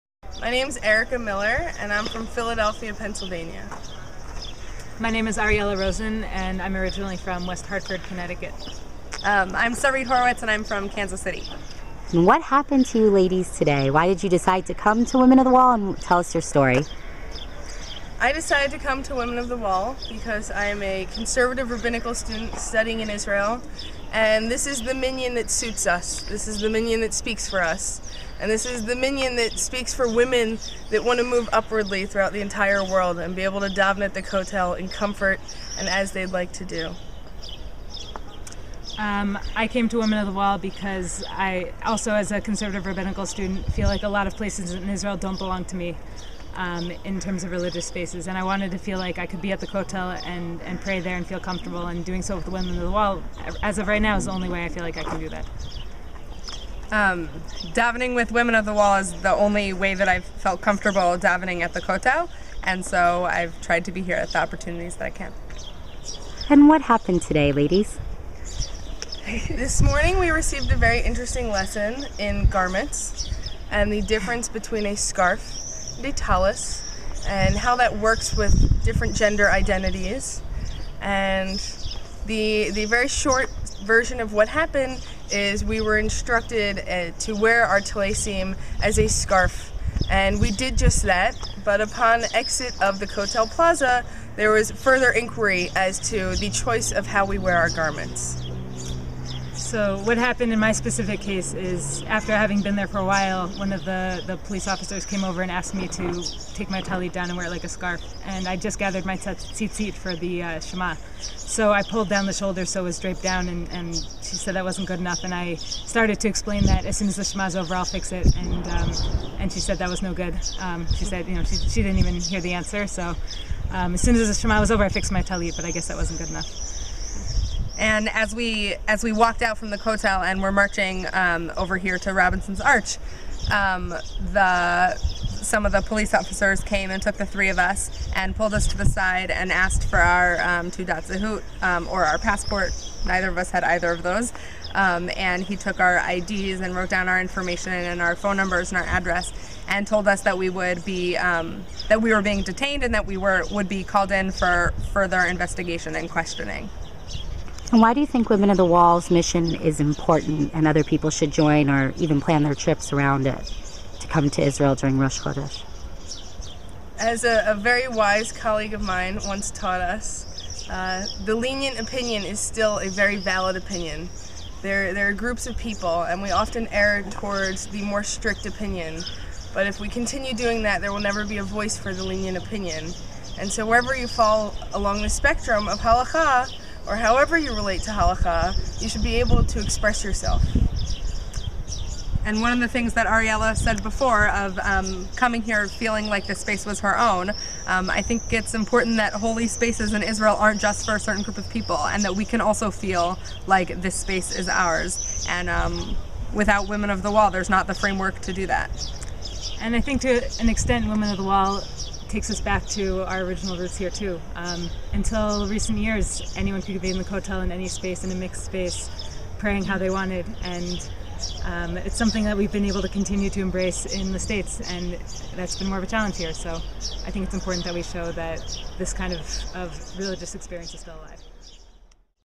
Rosh Hodesh at the Western Wall: No Prayer Shawls Allowed!
Check out this video of the three Rabbinical Students who were almost arrested at our Women Of the Wall Rosh Hodesh Sivan service today for wearing their tallit as a prayer shawl and not as a scarf.